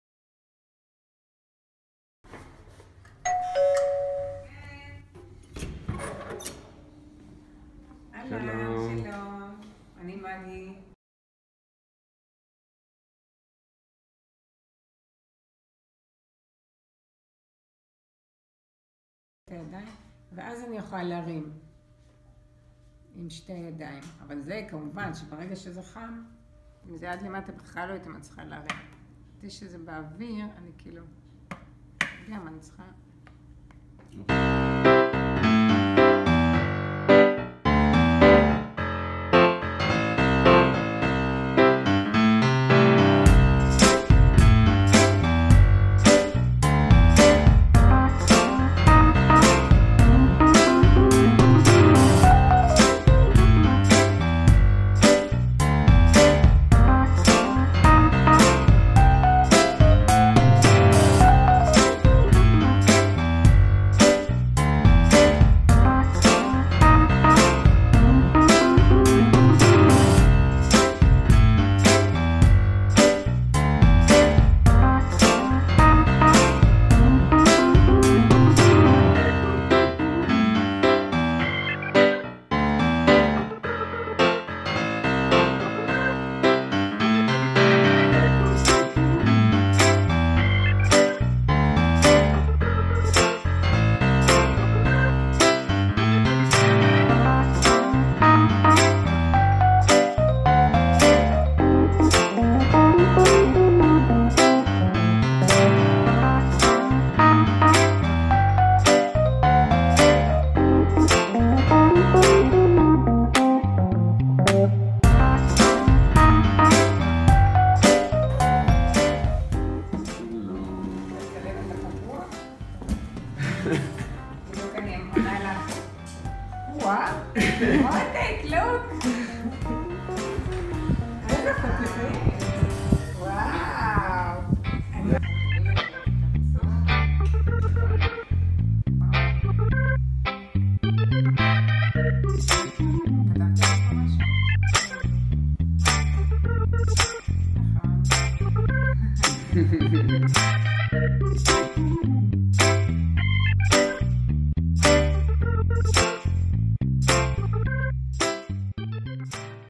Cup holder for Magi
Magi has impaired fine motor movements and is unable to feel temperate, so she struggles to drink hot tea and coffee. Fixperts from HIT come along to work on a solution.

Fixperts: Shay Alfia and Ori Cohen.
Fixpartner Margalit Eizner.
Educational institute: HIT.